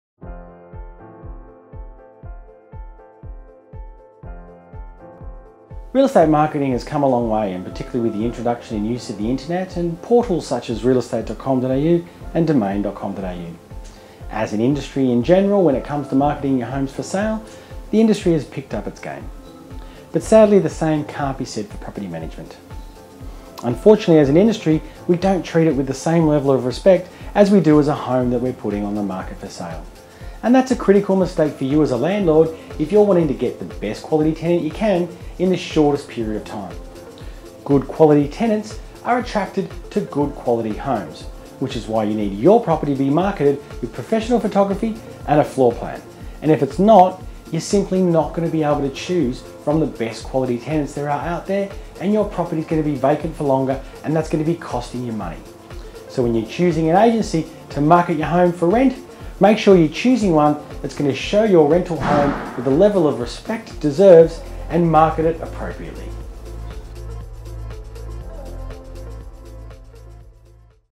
Marketing your home for rent - Troy Holmes
This week Troy discusses the important of properly marketing your home for rent.